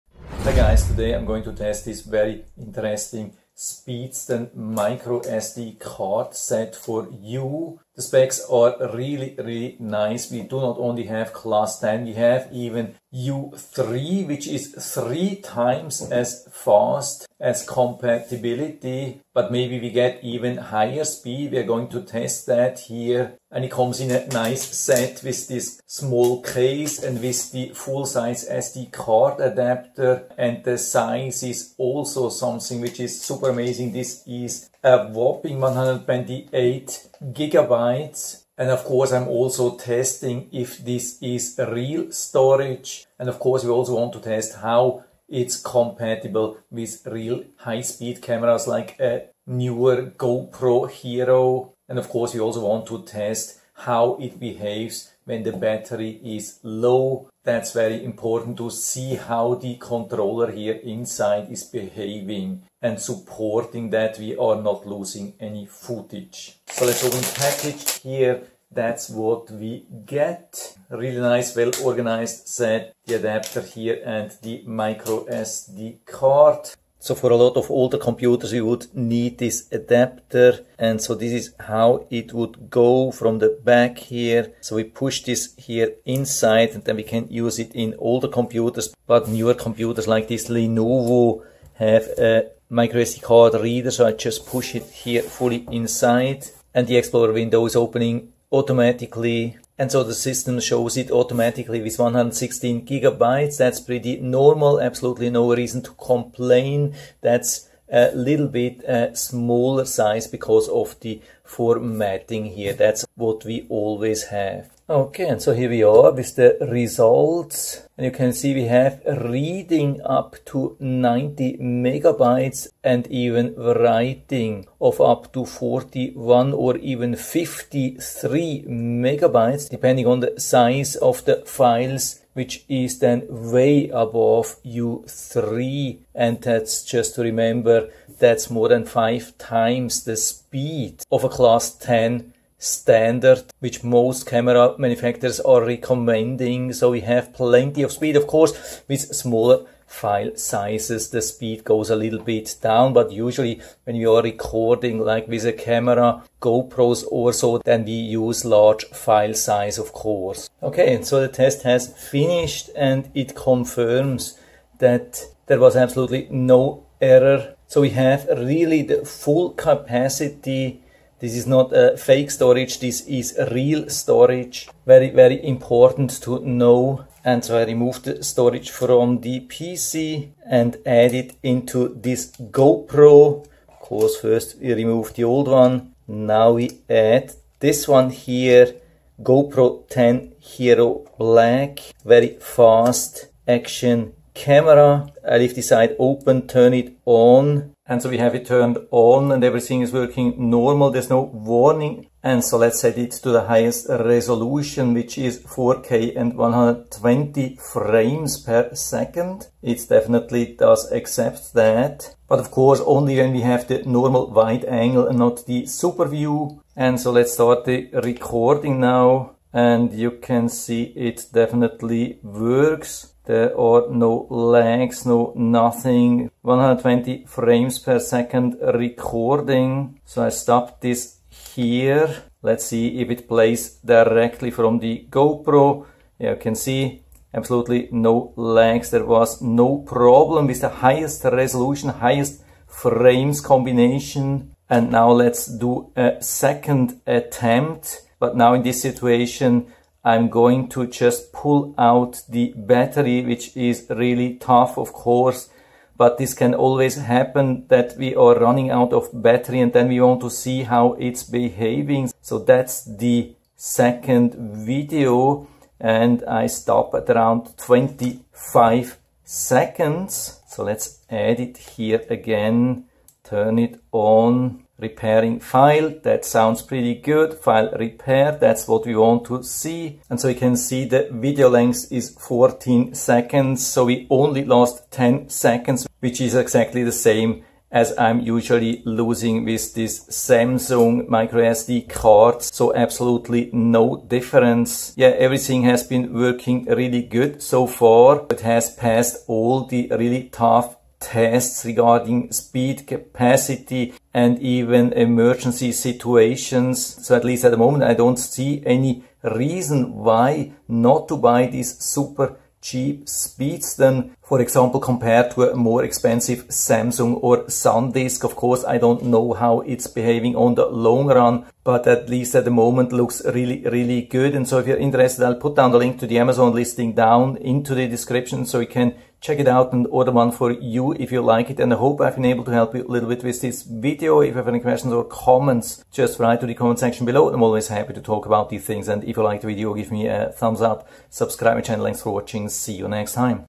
Speedston 128GB Micro SD Memory Card Full Review | Class 10 UHS-I U3 V30
In this video, I provide a full review of the Speedston 128GB Micro SD Memory Card, featuring 90MB/s read speeds, UHS-I U3 V30 rating, and Class 10 performance. Whether you're looking for a reliable storage solution for your camera, smartphone, or other devices, watch to see if this card meets your needs!

Speedston 128GB Micro SD Memory Card
SAMSUNG EVO 128GB microSDXC 130MB/s